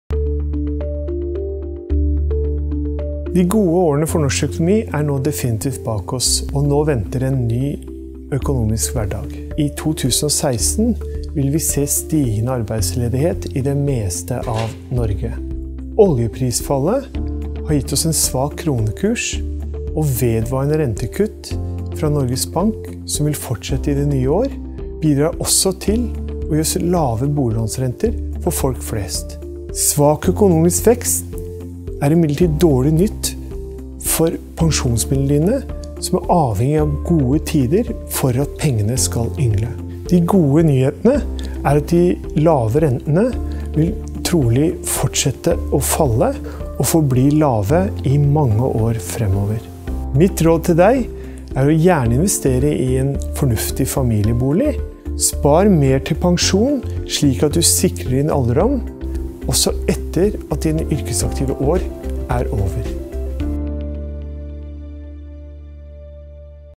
Økonomiske framtidsutsikter for 2016 m/sjeføkonom Jan Ludvig Andreassen
Etter noen svært gode år for norsk økonomi venter nå en ny økonomisk hverdag. For mange vil 2016 være året da de merker dette skiftet. Vi kan vente lavere vekst enn vi har hatt på mange år og vi ser nå også at arbeidsledigheten stiger. Se videoen med Sjeføkonom Jan Ludvig Andreassen og hans analyser om hva vi kan vente oss i 2016. Han har et viktig råd for din personlige økonomi: du bør sette av mer penger til pensjonen din.